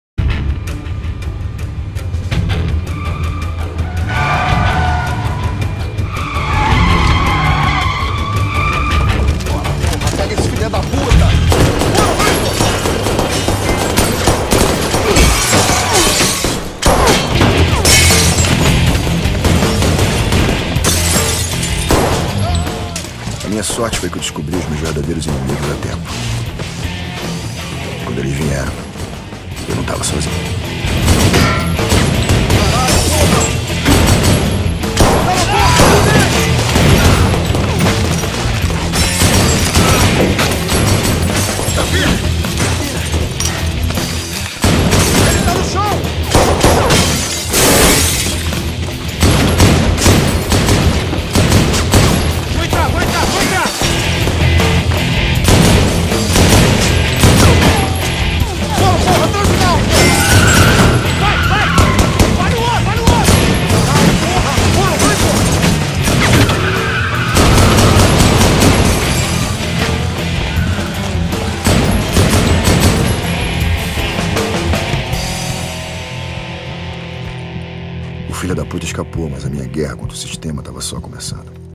Nascimento vs Rocha
Testativa de assassinato dos melicianos contra o Nascimento